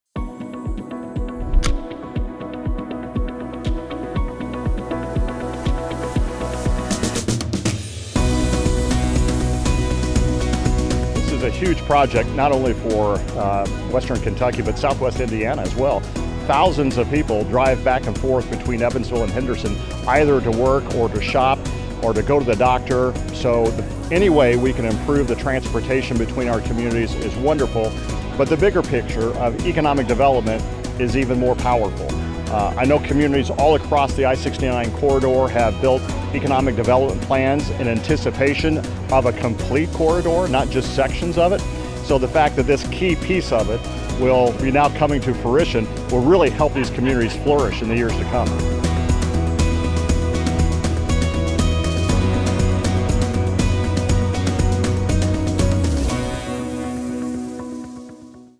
The Benefits of I-69 ORX
December 2022: Thousands of people drive back and forth between Evansville and Henderson to work, shop and more. Check out this short video with Evansville Mayor Lloyd Winnecke on the benefits I-69 ORX will bring to the area.